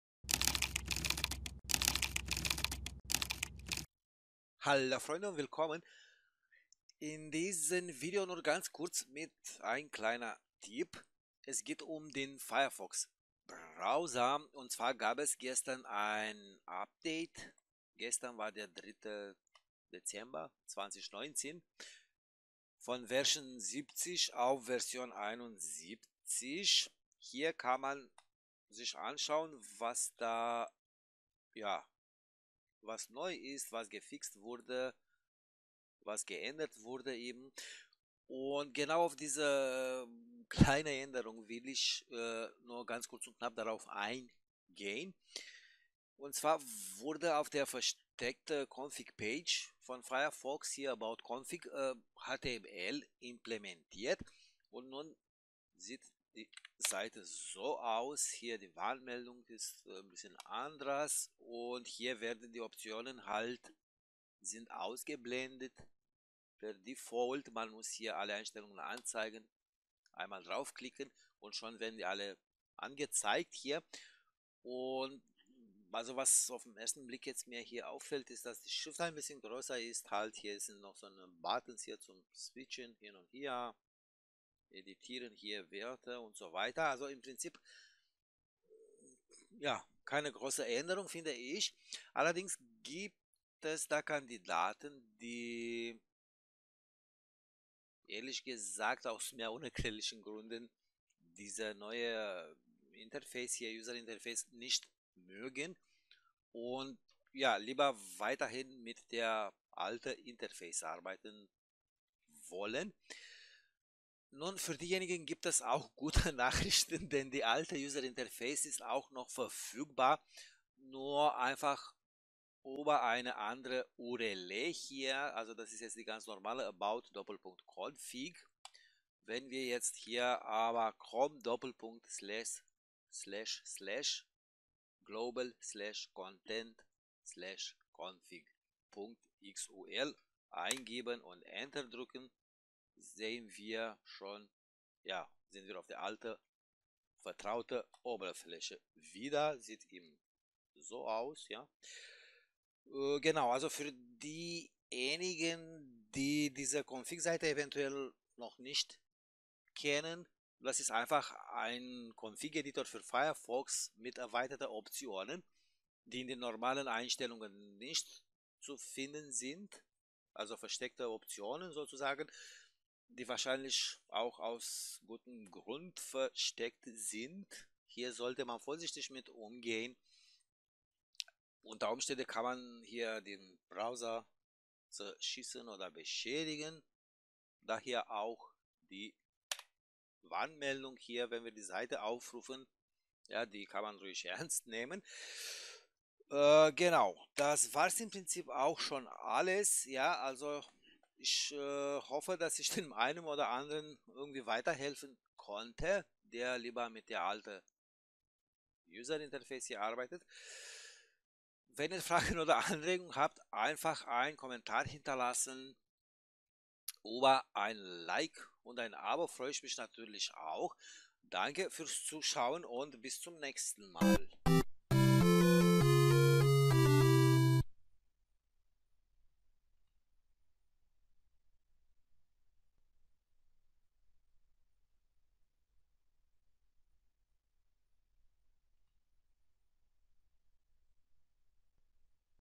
Firefox 71 Configuration Editor die alte Interface Oberfläche aufrufen | about:config
Neue UI      - about:config
Die alte UI  - chrome://global/content/config.xul

Gamer Stuhl :Robas Lund OH/FD01/NR DX Racer 1 Gaming-/ Büro-/  mit Wippfunktion Ergonomischer Chefsessel, schwarz-rot
bei Amazon / Partner - Link.